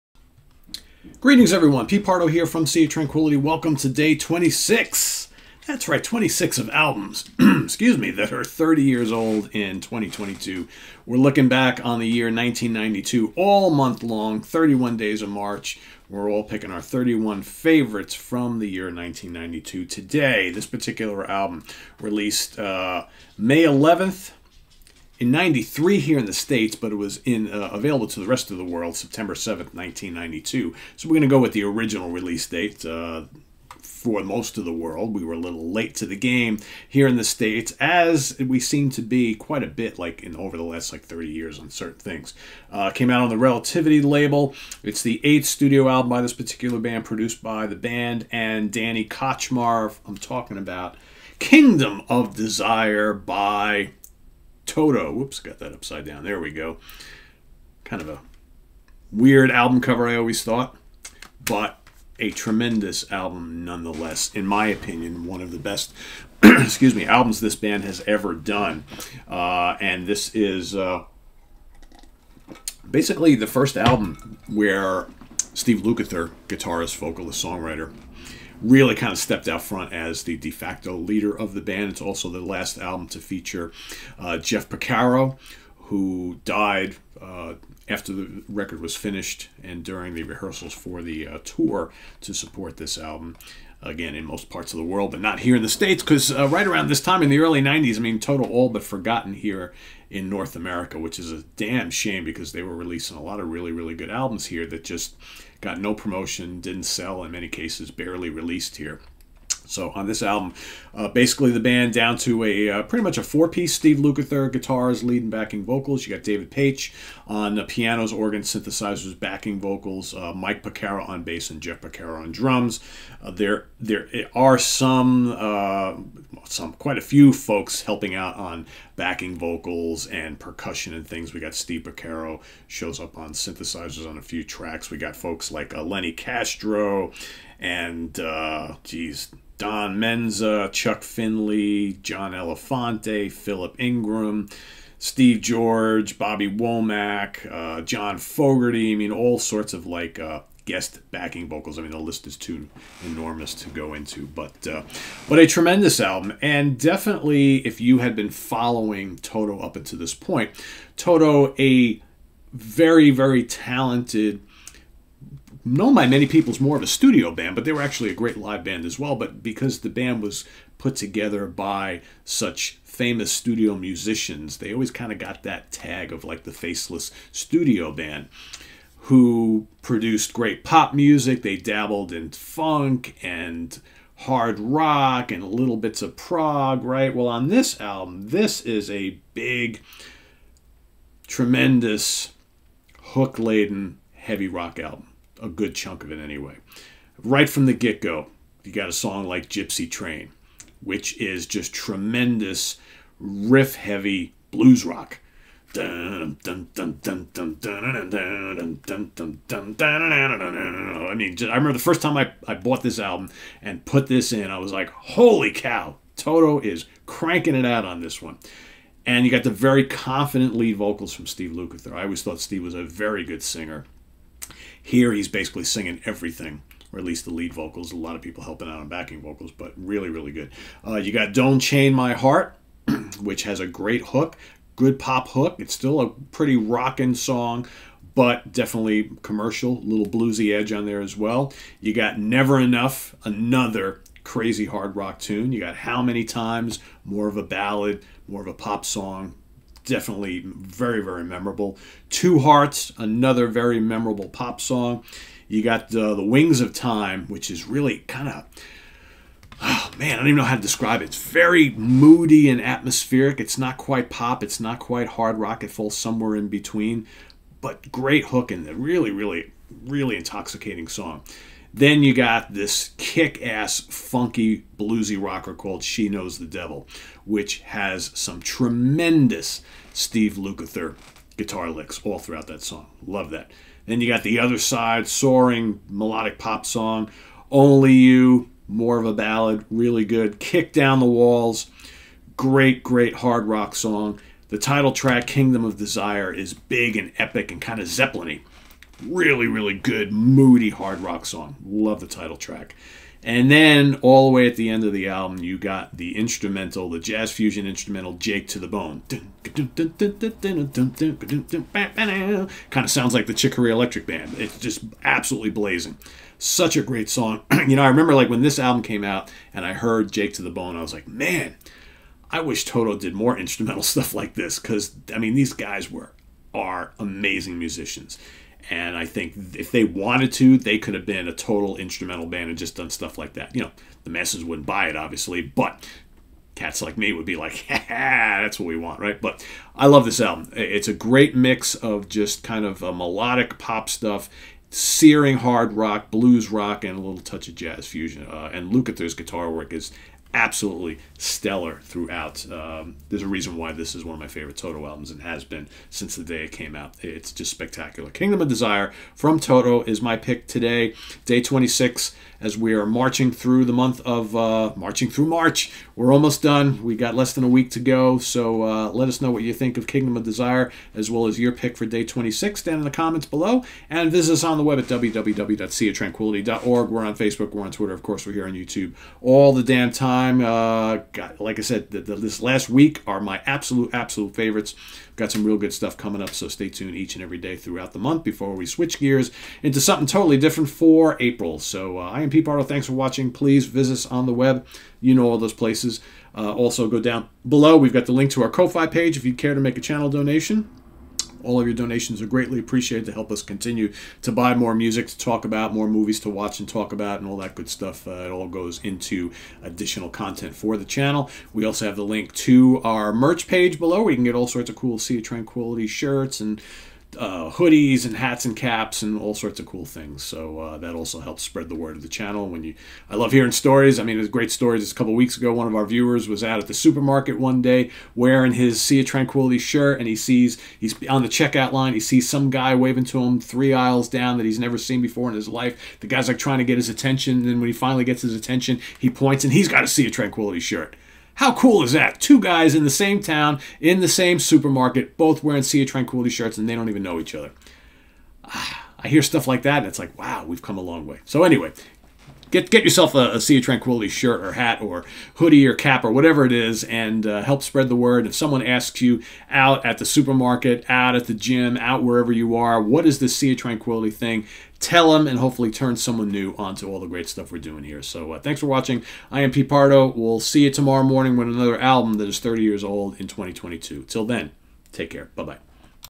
Albums That Are 30 Years Old in 2022: Day 26
Join Pete Pardo for a new series running each day throughout the month of March. Pete will be listing his 31 favorite albums that turn 30 years old this year from the magical year that was 1992, plus some honorable mentions of  releases that didn't quite make the cut at the end of the month. Please list one of your favorite albums that turns 30 this year from 1992 each day in the comments below.